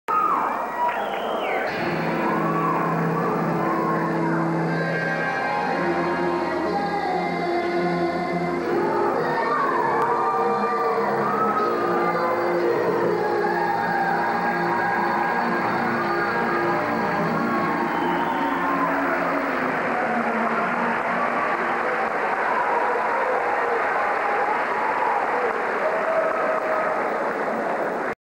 Anime Expo 2002 - #25 Fushigi Yuugi
Video of Entry 25, cosplay skit of Fushigi Yuugi, performed at Anime Expo 2002. (permission granted)
For other videos, be sure to check my favorites and please